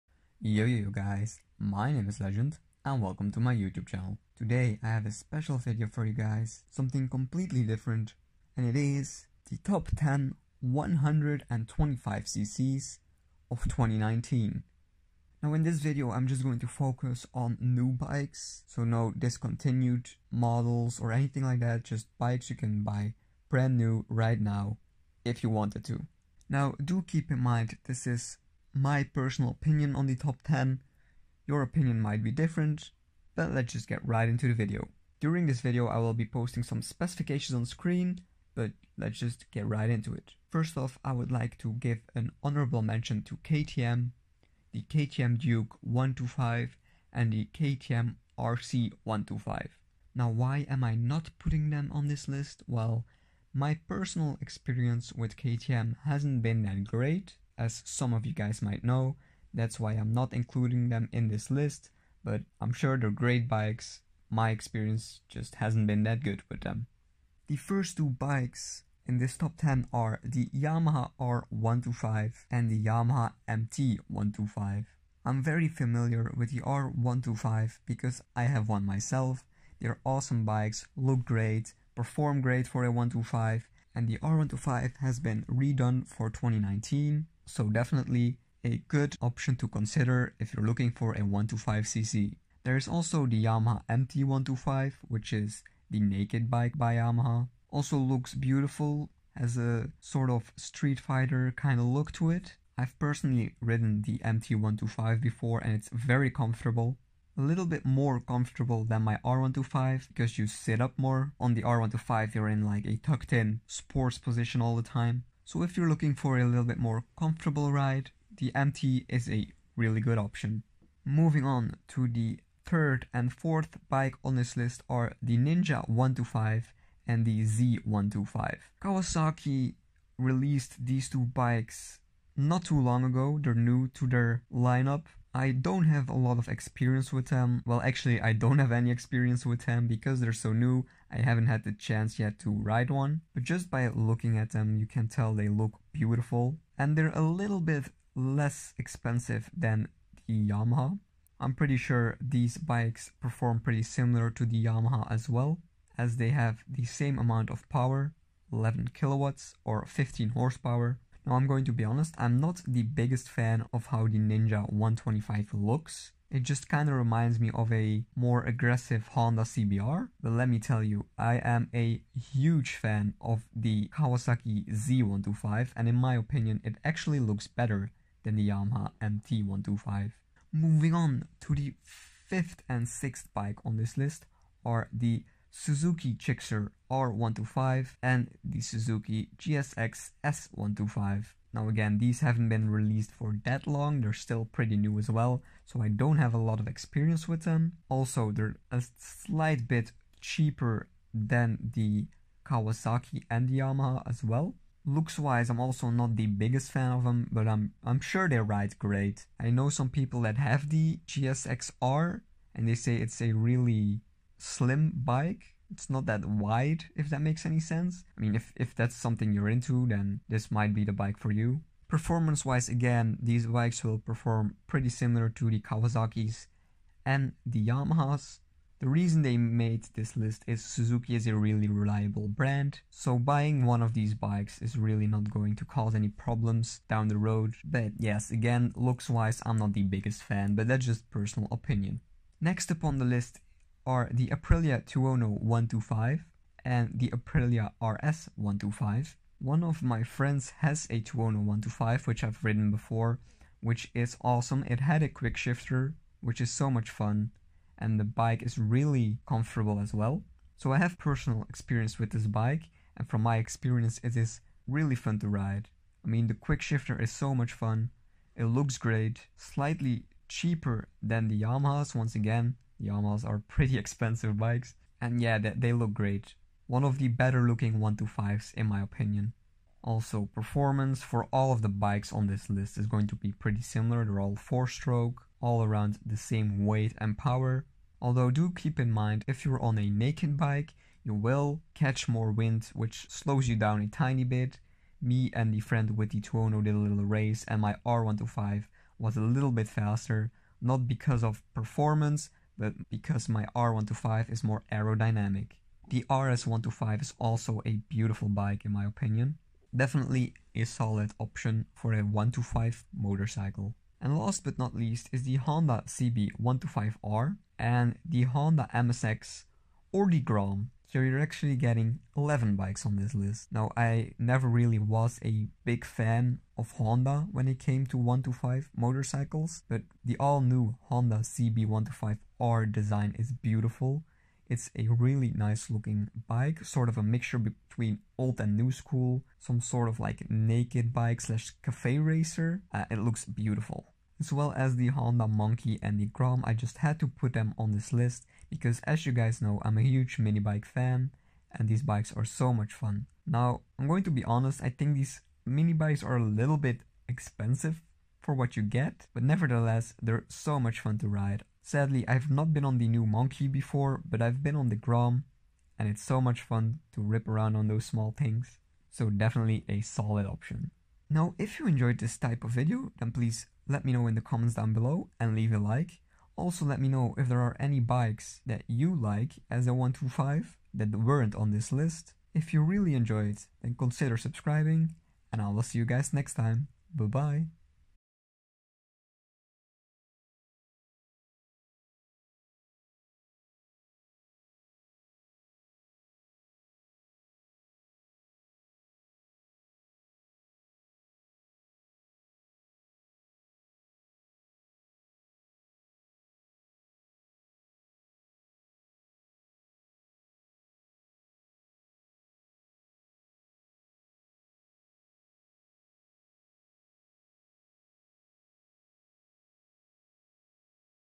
Top 10 125cc Motorcycles 2019
Let me know where you're from and which bike you are riding.
Feedback would be appreciated!

Hey guys, Today I share with you my top 10 125cc 4 stroke motorcycles for 2019. 5 different brands 2 bikes each enjoy!

bikes in this video:
Yamaha Yzf R125
Yamaha MT125
KTM Duke 125
KTM RC125
Suzuki GSX S125
Suzuki GSX R125
Kawasaki Z125
Kawasaki Ninja 125
Aprilia RS125
Aprilia Tuono 125
Honda CB125R
Honda Monkey125